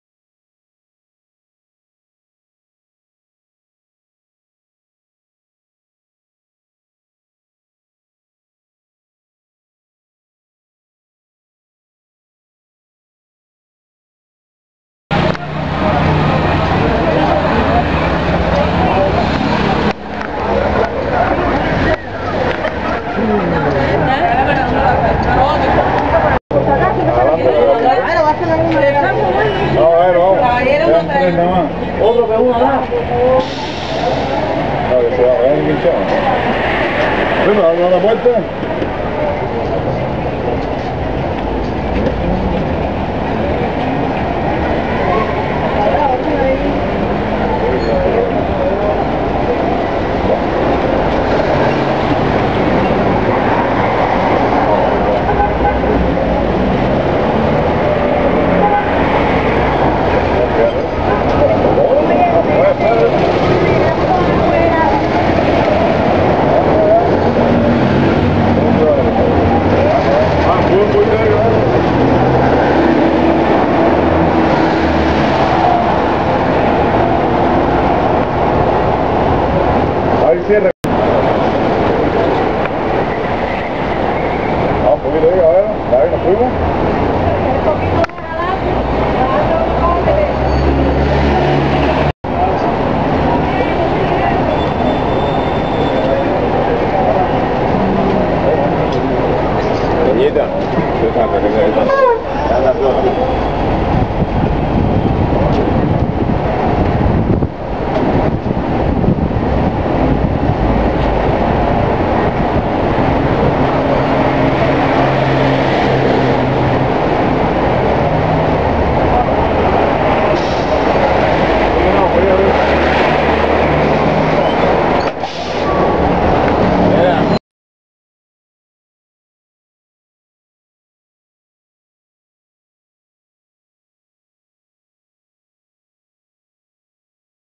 El P1 atrapado en Miramar
Un P1 atrapado por la masa que trata de llegar a sus deberes.